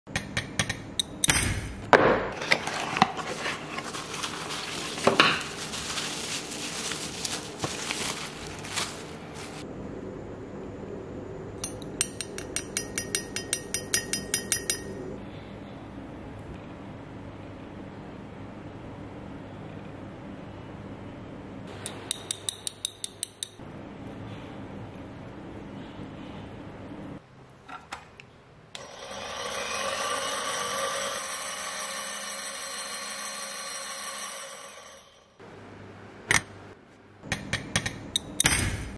3D printed metal gear every one can try | JLCPCB | The H Lab
First time try to order an 3D printed metal gear. Everyone can try with below link. jlcpcb now provides 3d printing service. Not only metal but only resin, abs, nylon... Let's see how good it is.